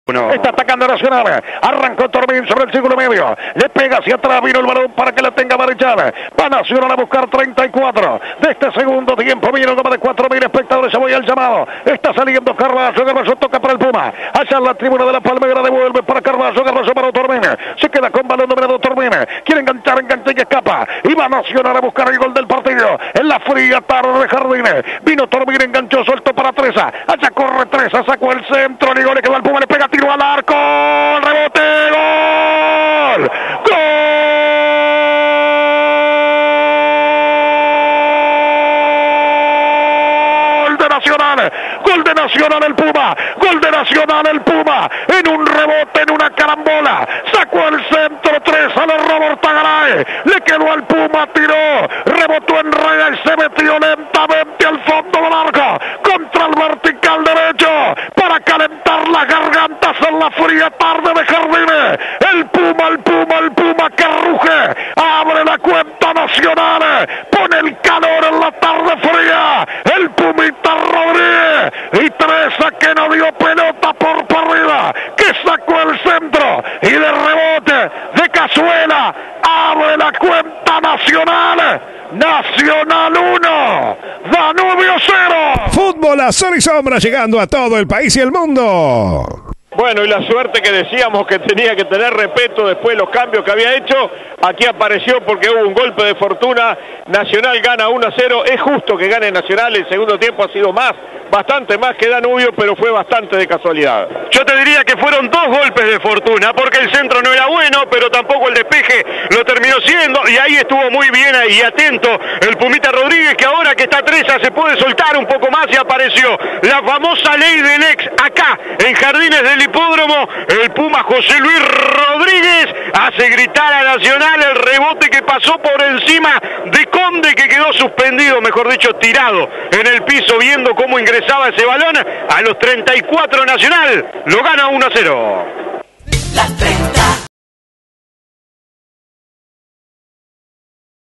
Danubio 0 Nacional 1 - Intermedio 2022 (por Eduardo Gutiérrez)
El gol del partido, por Eduardo Gutiérrez junto a Daniel Ordóñez y Alfonso Irrazábal para CX 30 Radio Nacional 1130 AM.